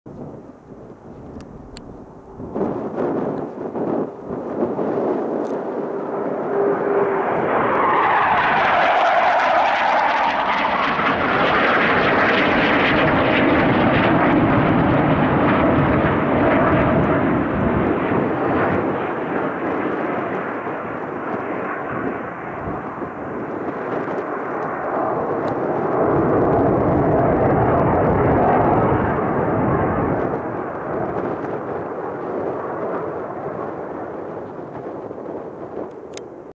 Typhoon from Warton Aerodrome low level training in the Lake District UK.
Just a short video of a Typhoon from Warton Aerodrome flying along Thirlmere in the Lake District. This was a new location for me just to see if there was any benefit. I did catch it a little late as it was murky and overcast at times however I think it has promise. albeit with a long walk to get there!!